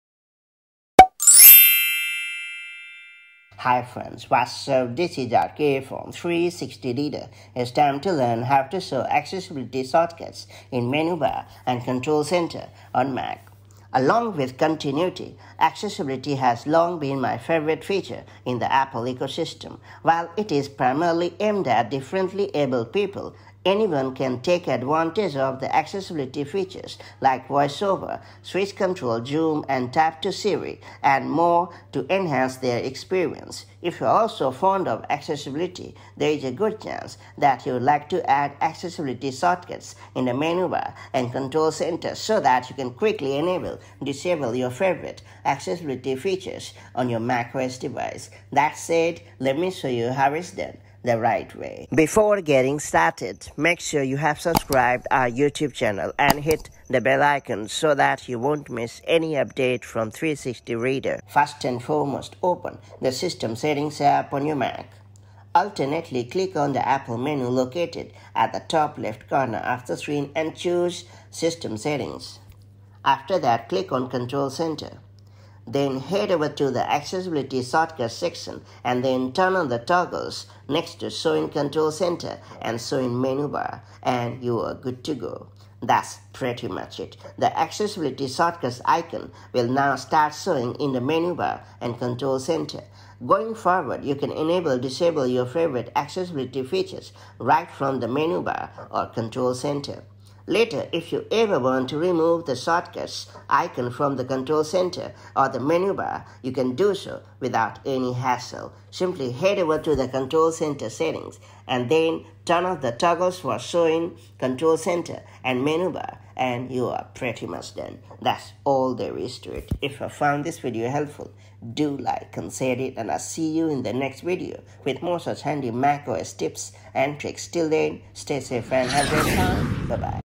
How to Show Accessibility Shortcuts in Mac's Menu Bar in macOS 13 Ventura
Here is how you can display or add Accessibility Shortcuts to the menu bar on Mac in macOS 13 Ventura. Let's find out!